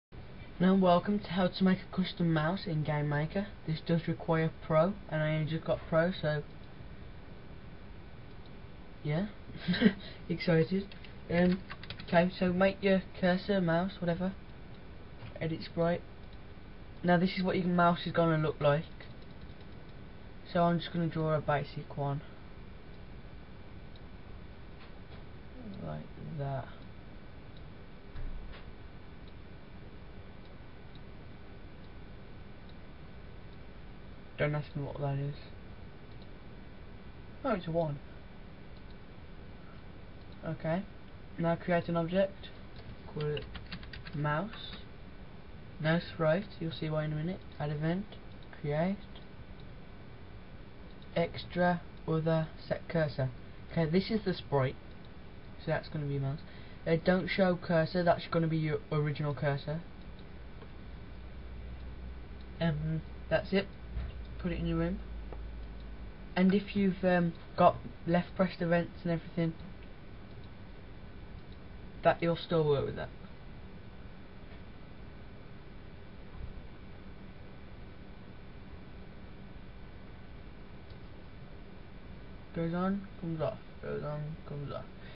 Game Maker: Custom Mouse
Make Your own mouse! Sorry pro edition required:(